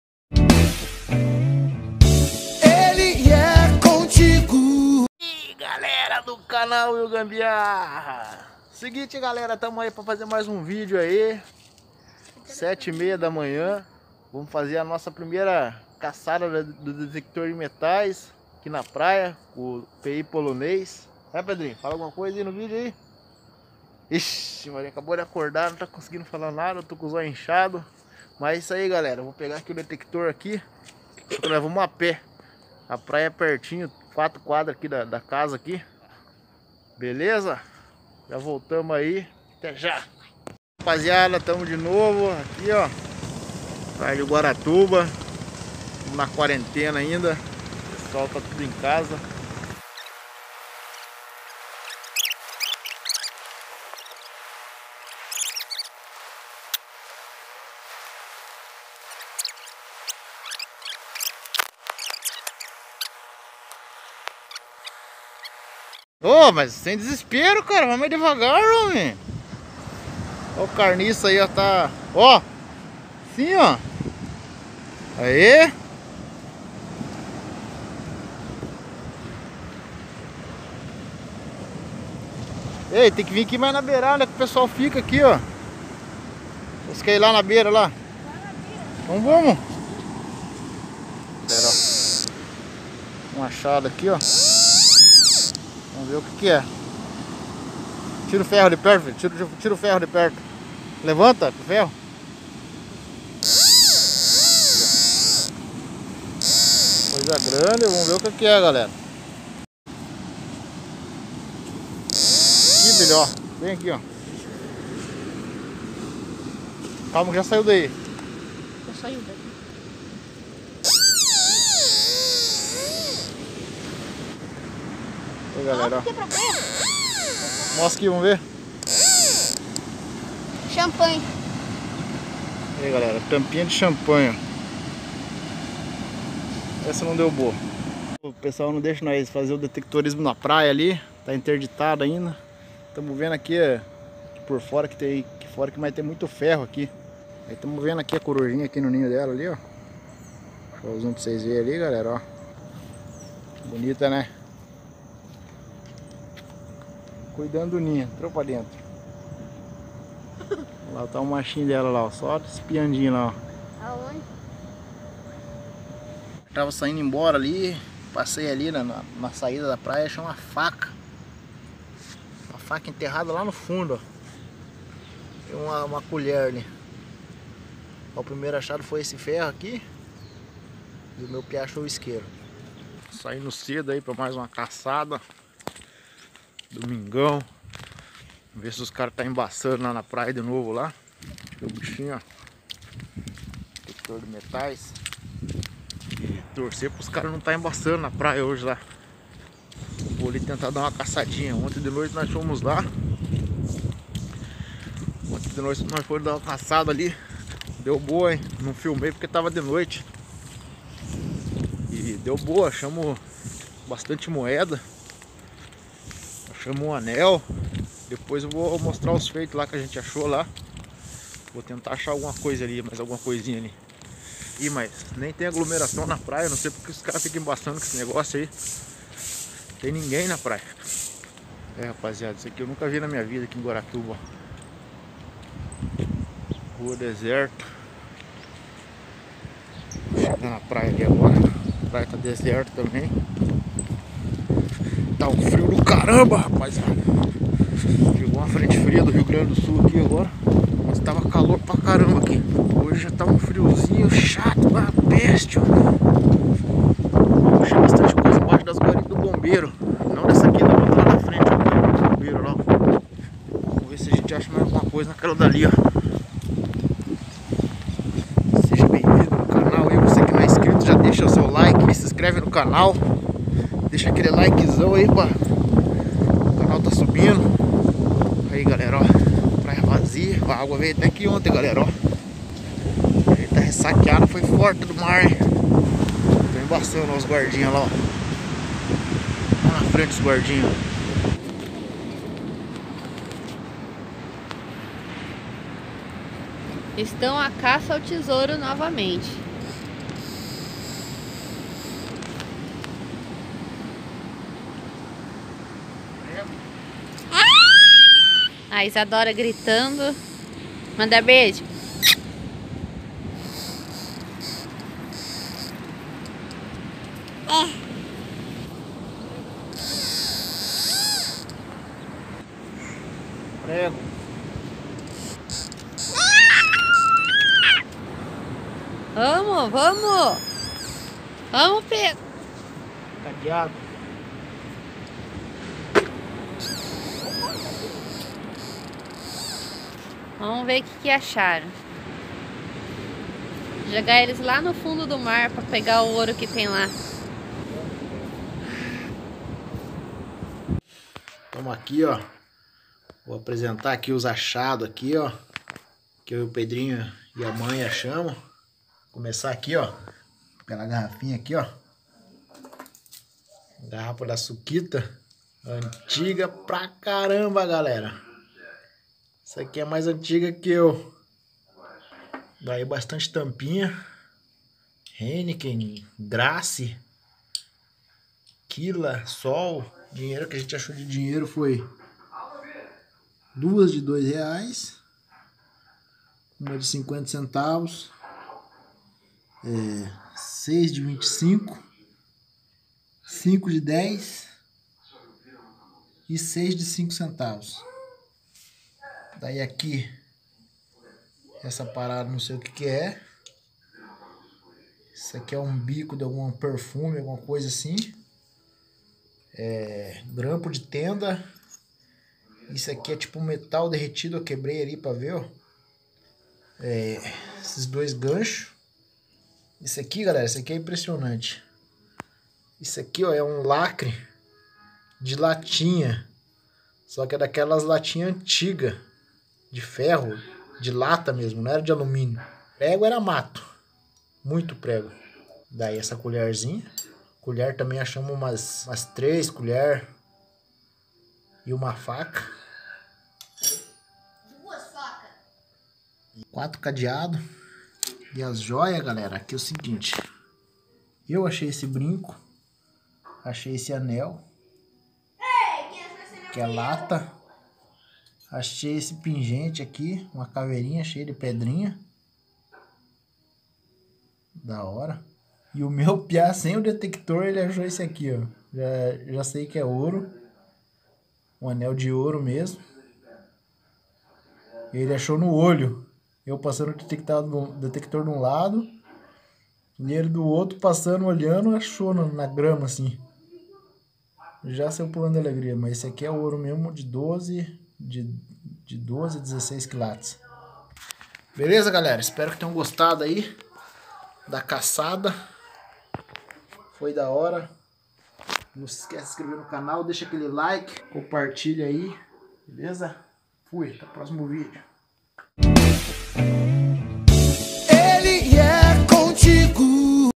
Primeira caçada com pi polonês
Nesse video fizemos uma caçada com pi polonês na praia de guaratuba por tres dias mas poucas horas de detectorismo, pois as praias estavam enterditadas ...vários achados como moedas e jóias...